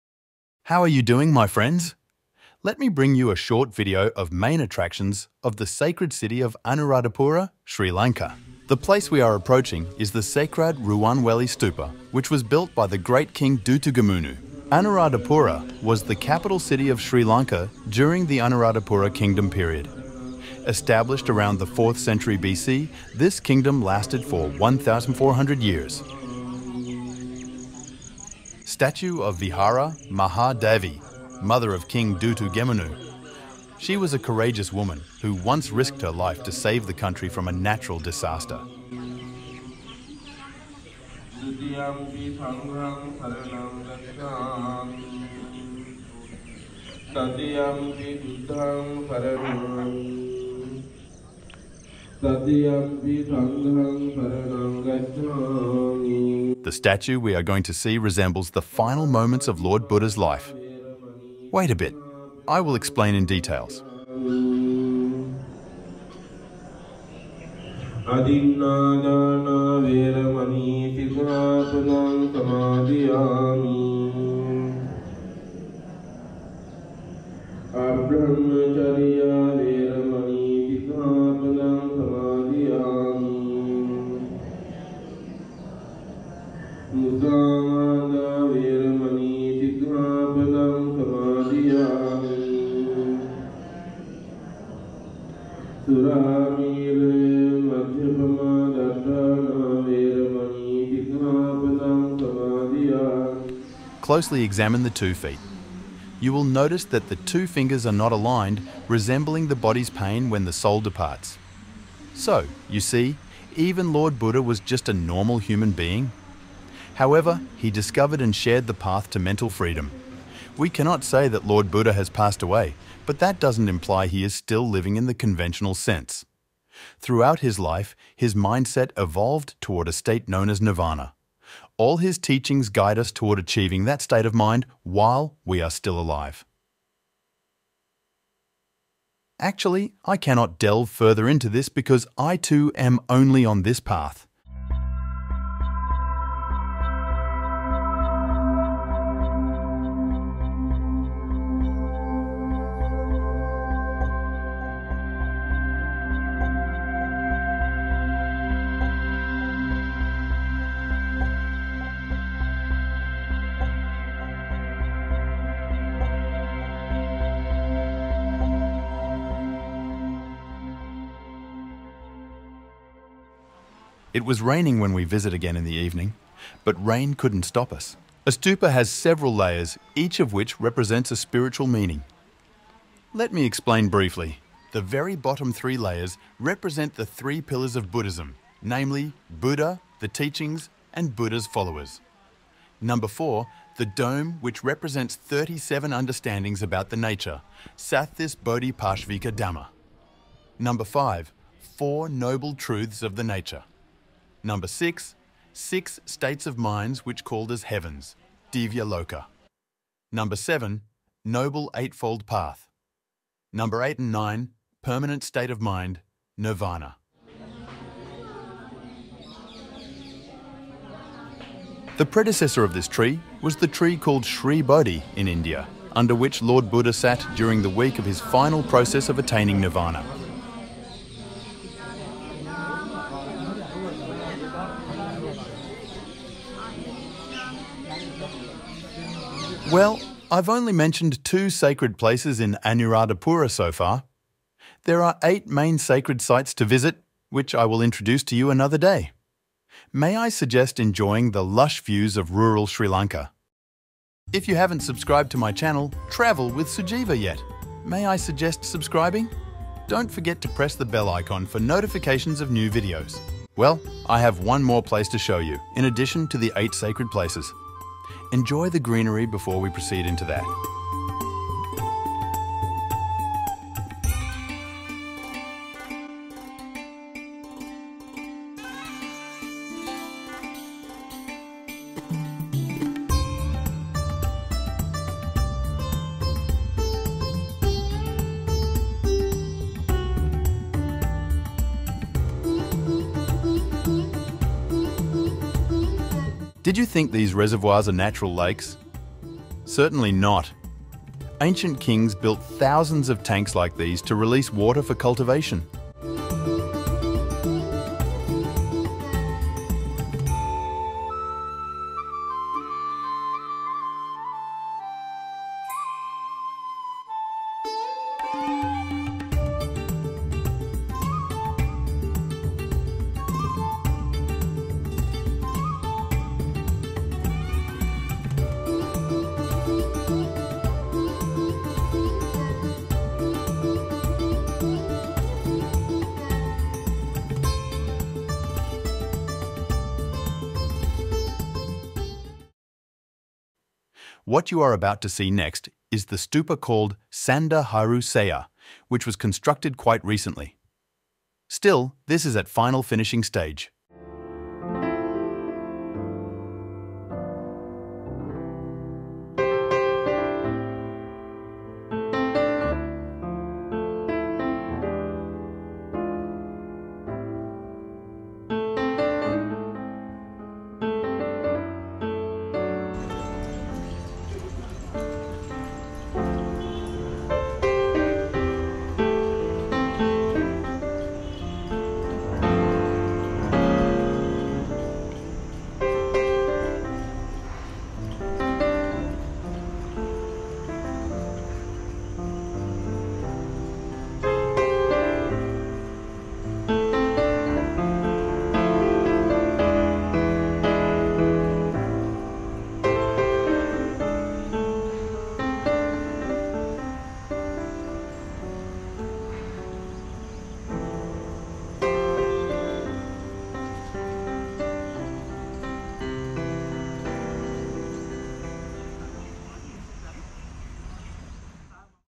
Ruwanweli Seya | Stupa | රුවන්වැලි මහා සෑය | Anuradhapura | Ancient | Sacred | City | Sri Lanka
Sacred City of Anuradhapura
This sacred city was established around the 4th century BC. The kingdom lasted for more than 1500 years. Due to the richness of the country, Sri Lanka was a target of greedy invaders from both eastern and western world. As a result, the kingdom was abandoned.

Among the influences, buddhism is the jewel Sri Lanka got from eastern world. Anuradhapura sacred city is the best example in Sri Lanka to show the Buddhism induced richness (physical and mental) of ancient Sri Lankans.